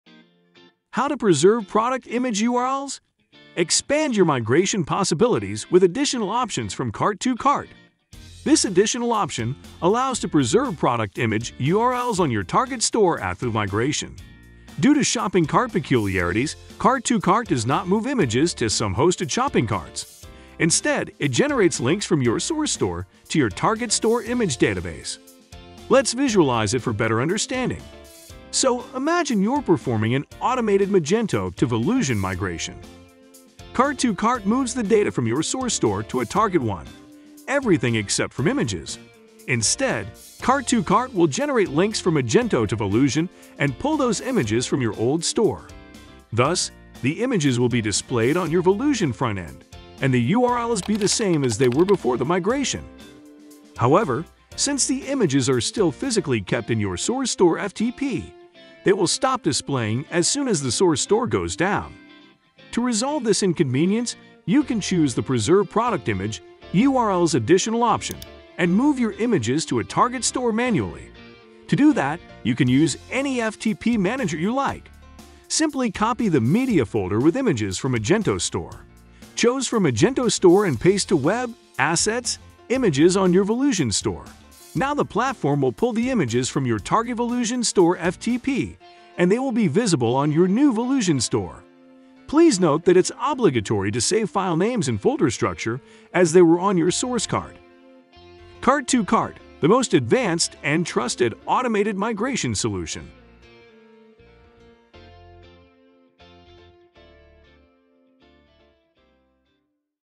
How to Preserve Product Image URLs when Migrating with Cart2Cart?
Preserve product image URLs on your Target store after the migration! On example of Magento to Volusion migration, the video demonstrates how and what does the “Preserve product image URLs” additional option do.

Basically, the option allows you to preserve product images URLs so that they’ll be pulled from your Target store database instead of the Source one. To do it, simply choose the additional option at the page on entities selection and launch the migration. Afterward, you’ll have to copy the images to your Source FTP to Target manually.